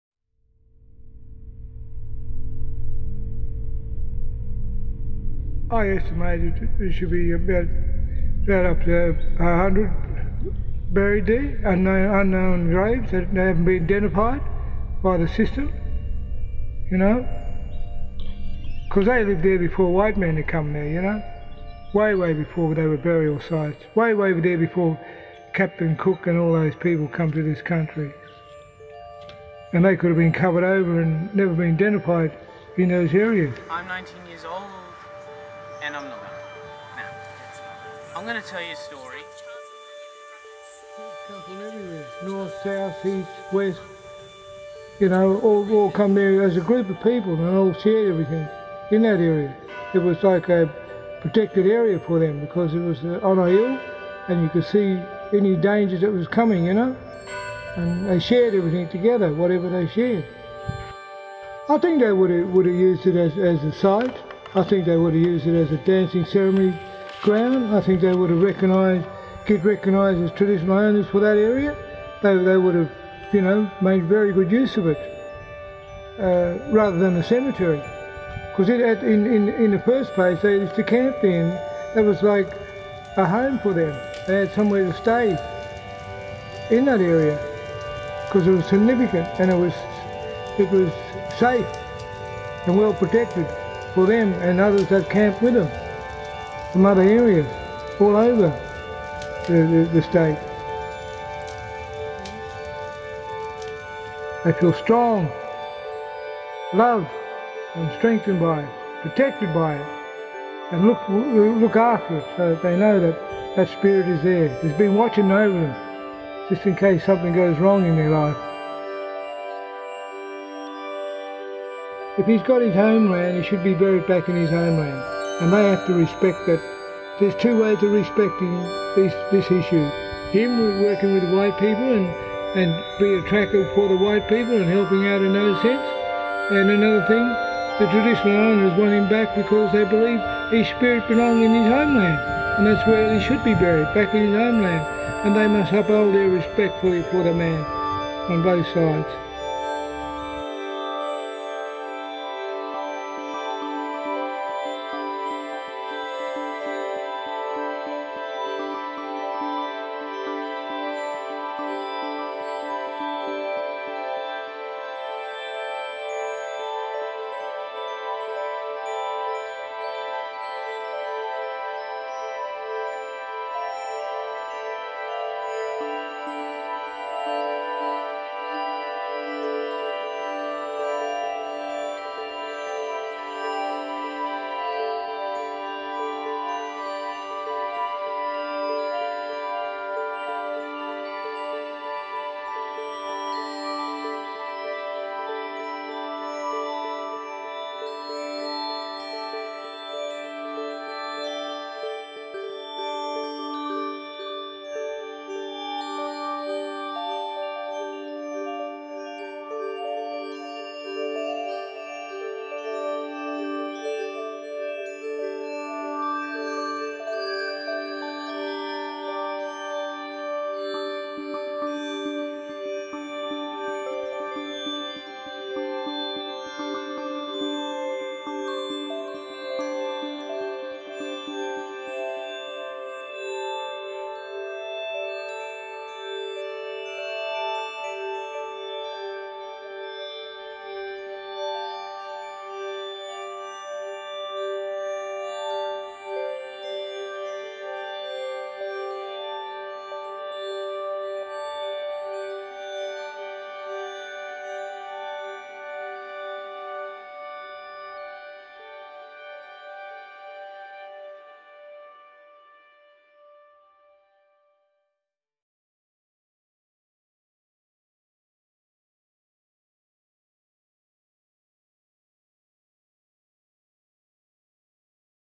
REST - East Perth Cemeteries
REST is an immersive promenade theatre experience presented at East Perth Cemeteries in 2019. The result of a collaborative partnership between the National Trust of Western Australia and the WA Youth Theatre Company it shared the diverse and extraordinary histories of this significant heritage place with new audiences through site specific performance.